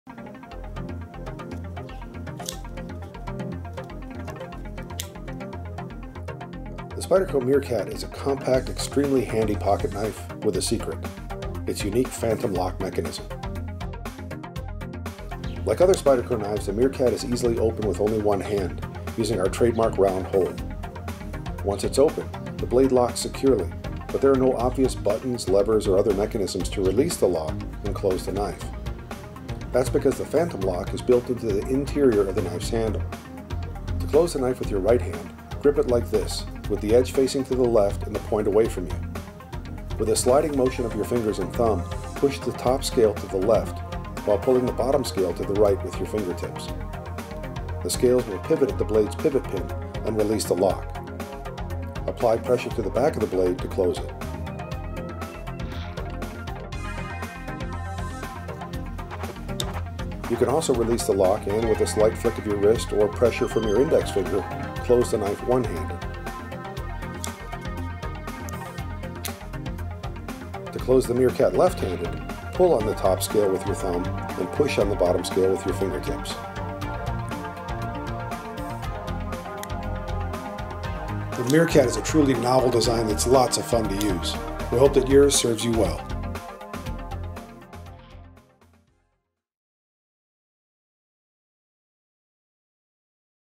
Spyderco Meerkat Phantom Lock Operation - How To
Here's a quick look at how to operate the Spyderco Meerkat Phantom Lock

Thank you for your continued trust, confidence, and interest in Spyderco and our products. We are honored to serve you.